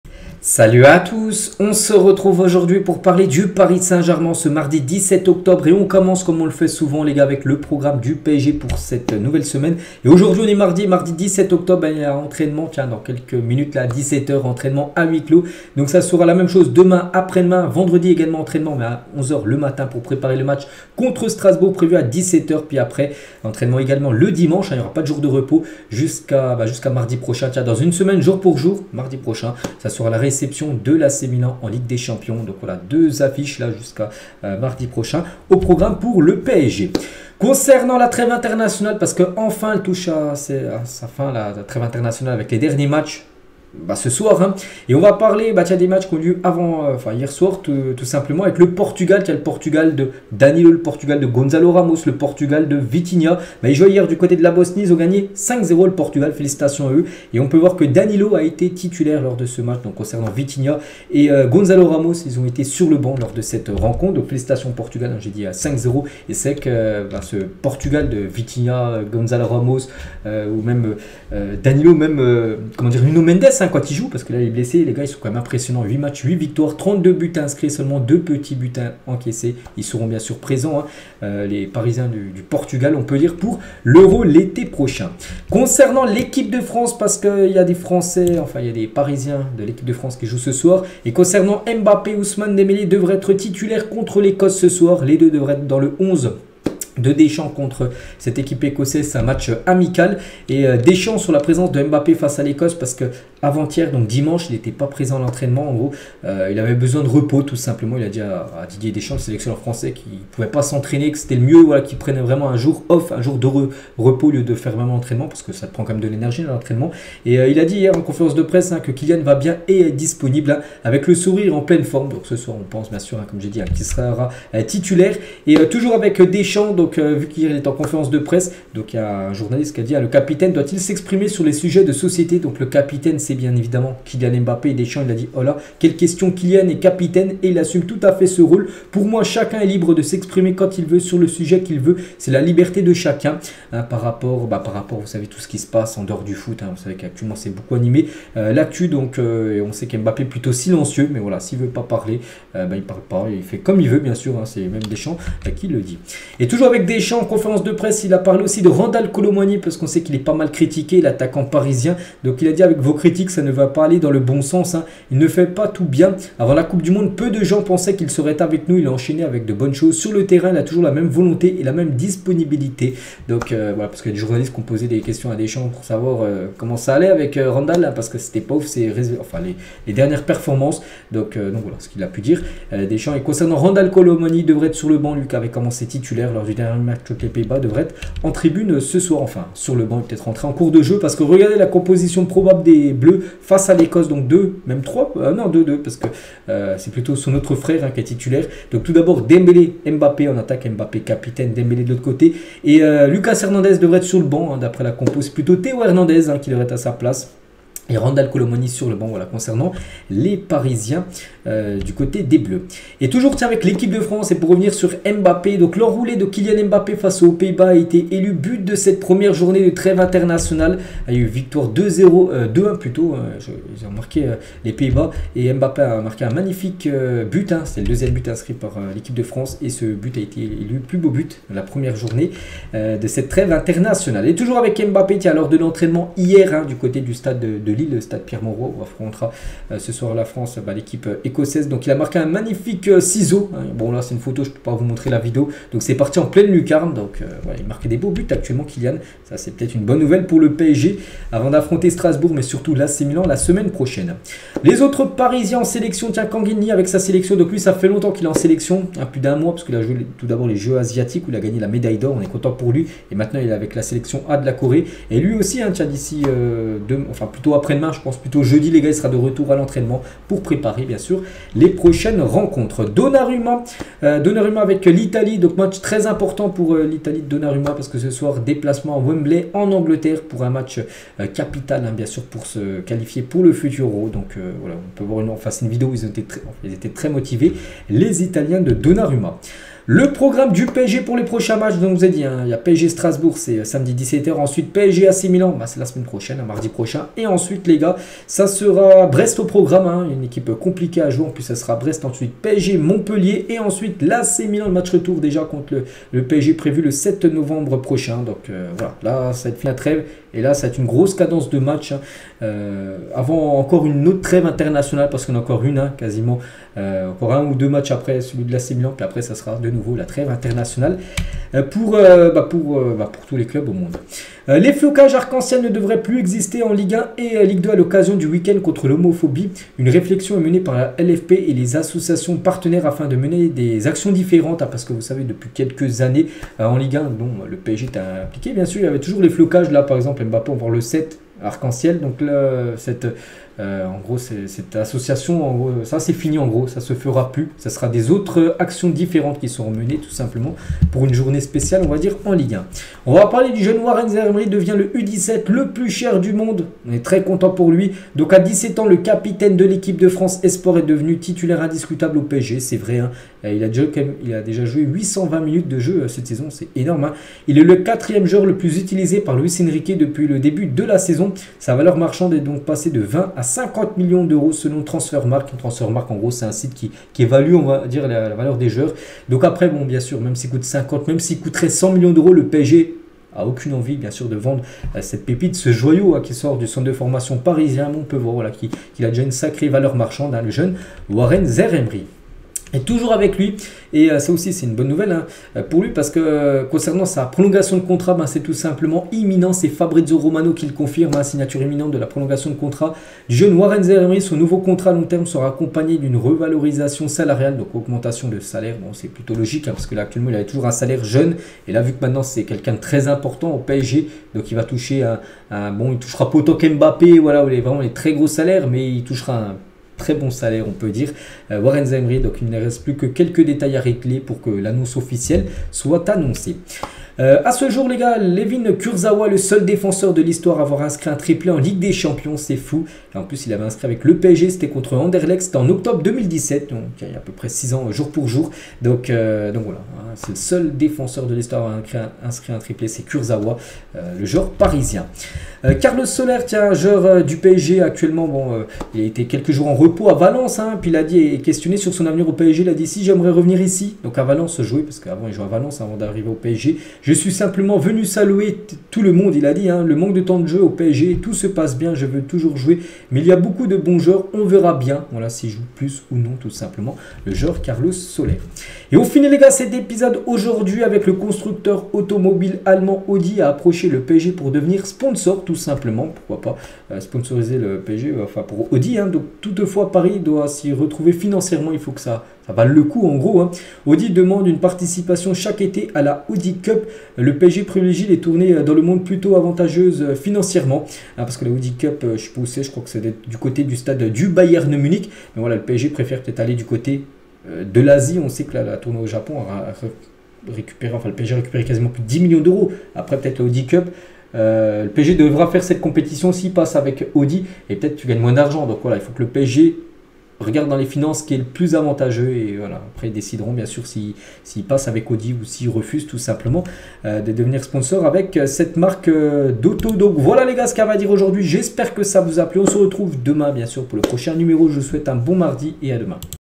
TRES BONNE NOUVELLE POUR LE PSG AVEC WARREN ZAIRE EMERY ! / AUDI A APROCHÉ LE PSG !
Bonjour a vous
Ici on parle de l'actu du PSG.
Bonne journée